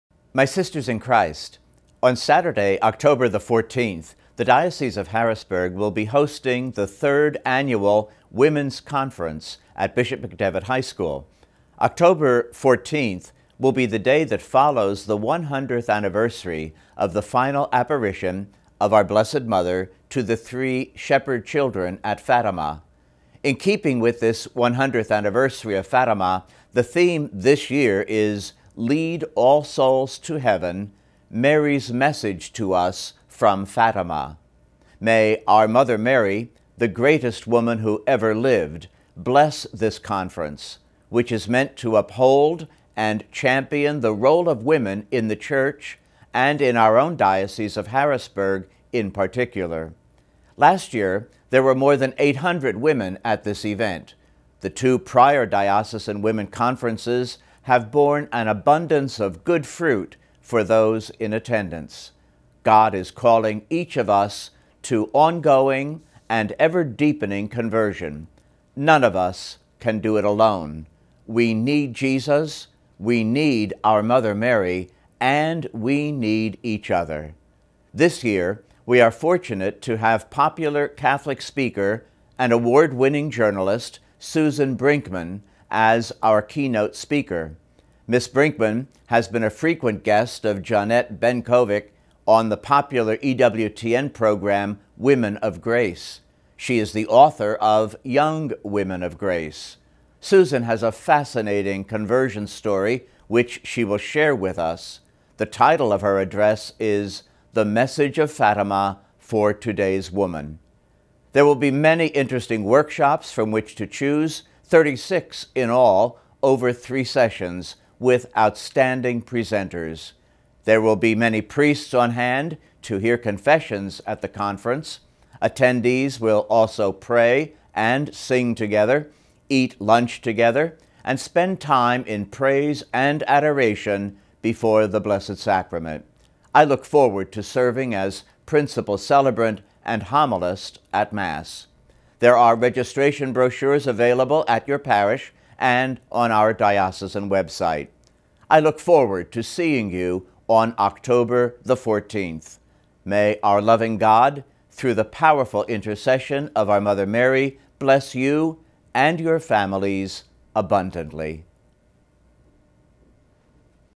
Women's Conference 2017
The Harrisburg Diocesan Women's Conference will be held on Saturday, Oct. 14, 2017. Bishop Ronald Gainer offers this invitation to the day long event.